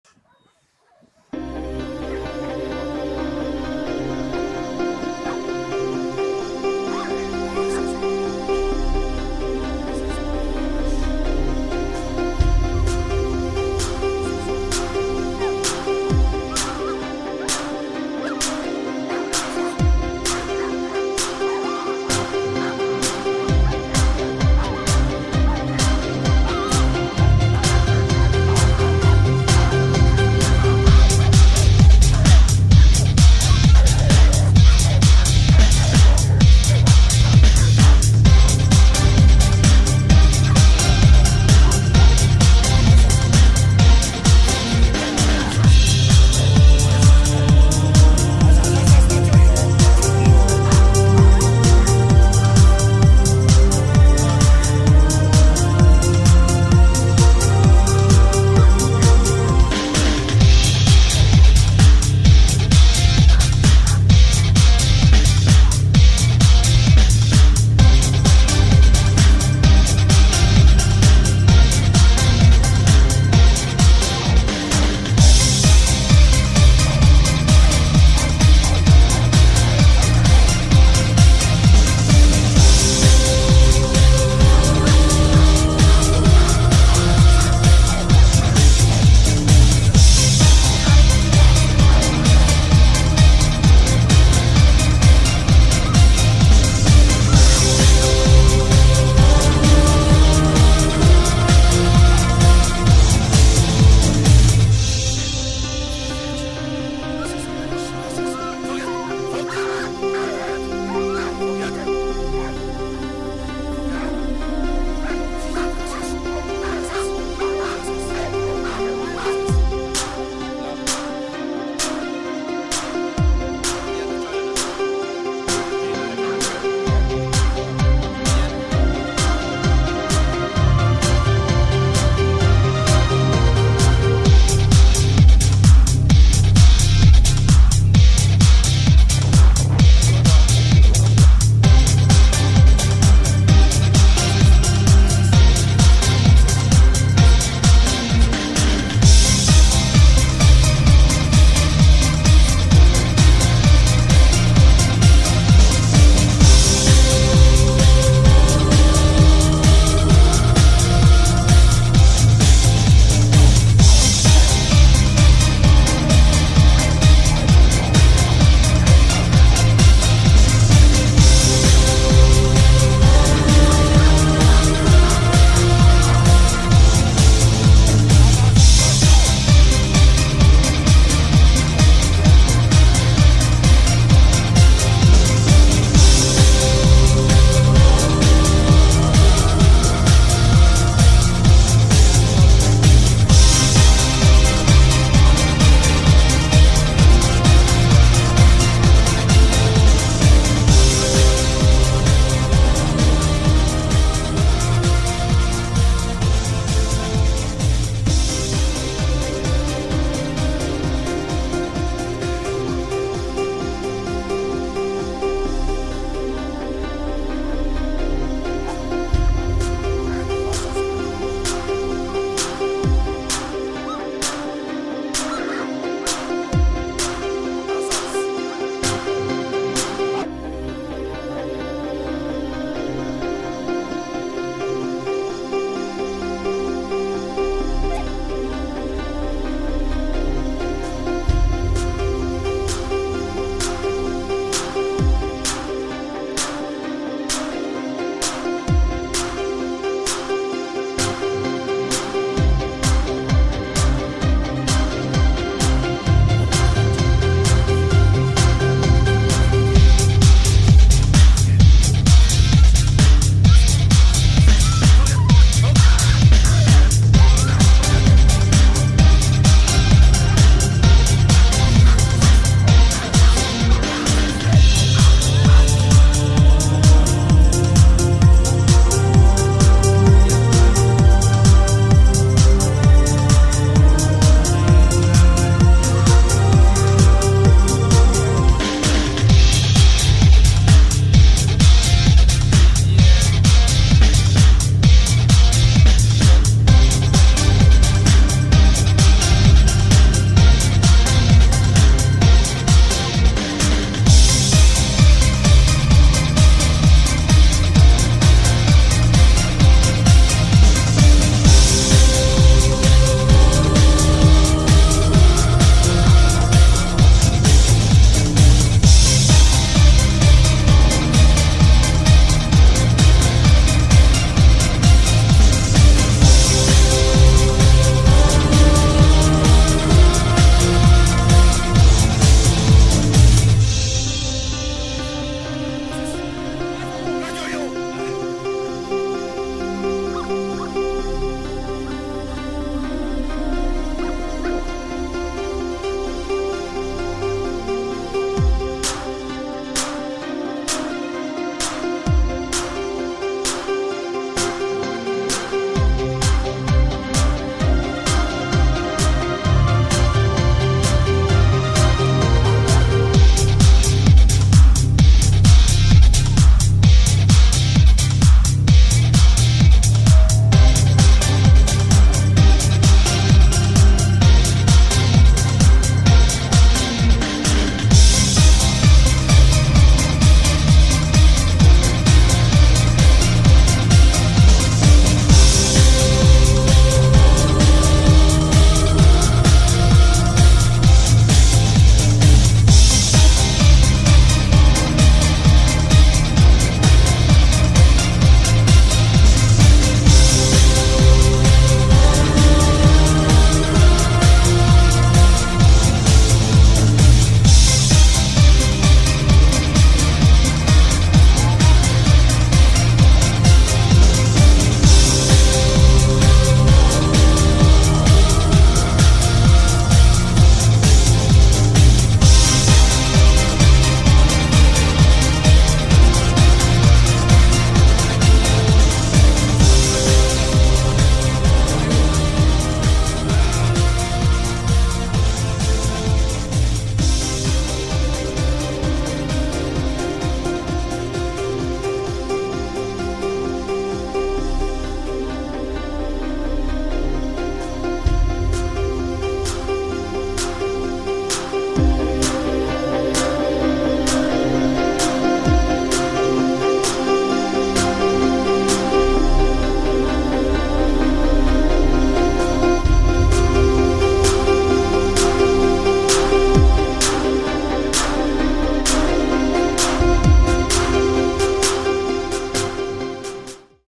20190512 TDT kutyasuli
Rottweilereink az őrző-védő képzés kezdő csoportjában.